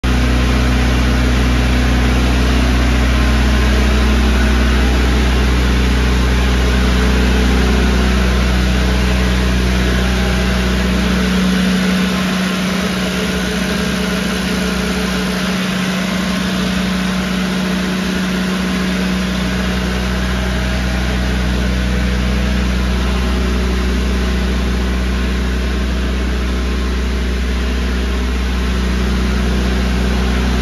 Lot 124 - 40' Conex Box with Workshop & Generator - Auction Aug 15 at 10AM. Details at
40 Conex Box Work Shop w/ Onan Diesel L423 Generator w/ 2,251 Hrs., 220 and 408V, Ingersoll Rand Air Compress T30, Exterior 220V Power Box, and 250G Diesel Tank